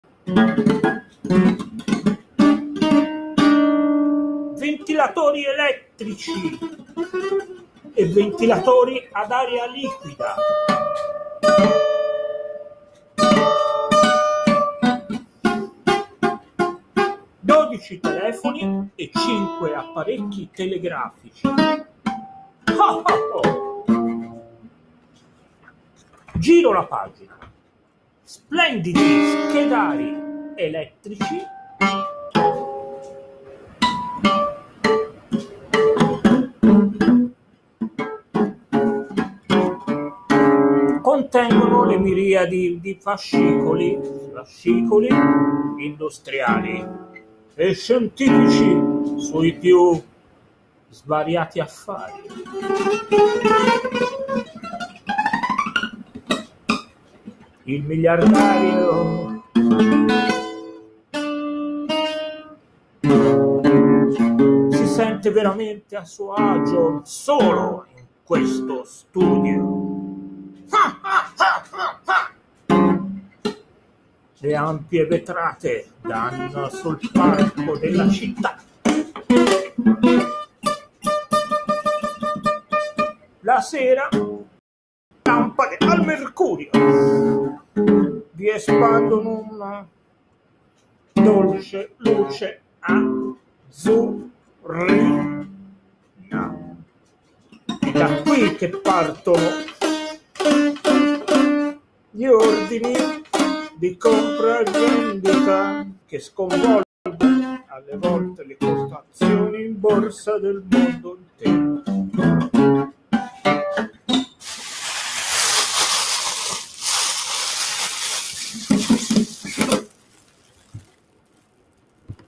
Eugenio Sanna- lettura con improvvisazione musicale di Office di Blaise Cendrars
Eugenio Sanna- acoustic guitar, voice, cellophane giallo
25 aprile 2020: lettura liberamente tratta, trattata, con musica improvvisata, della poesia "Ufficio" di Blaise Cendrars, dai Documentaires